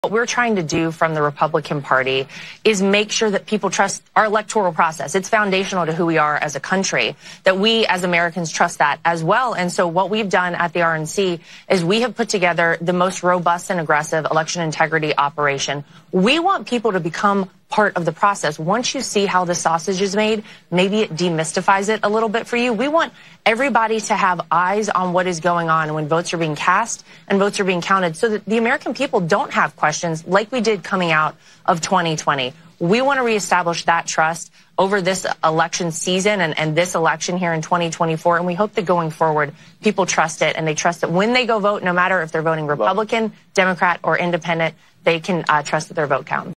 The RNC’s robust election integrity program is working to ensures your vote counts!
The RNC’s robust election integrity program is working to ensure that “when [Americans] go vote, no matter if they’re voting Republican, Democrat, or independent they can trust that their vote counts” - RNC Co-Chair Lara Trump